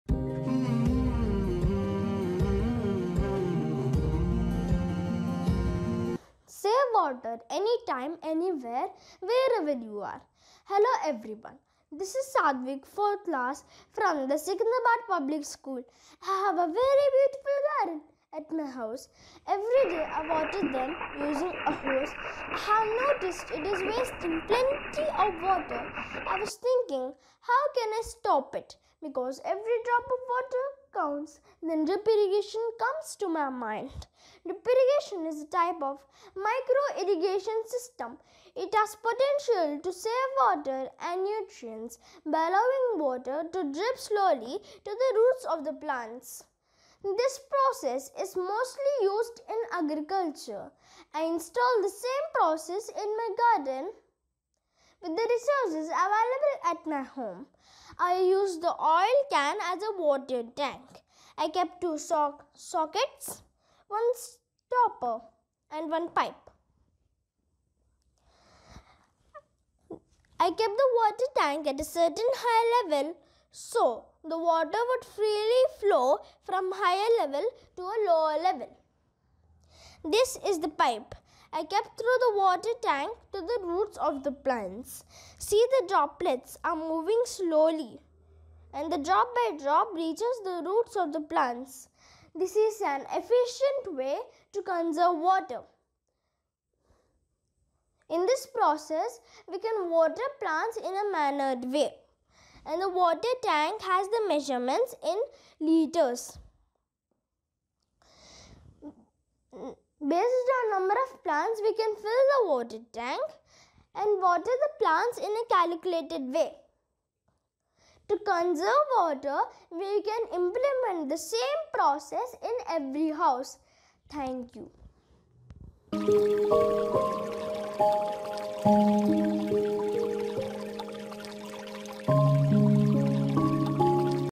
Science and Maths - Drip Irrigation process for house gardening By sadhvik
Most attractively described the process of Dripirrigation for house gardening.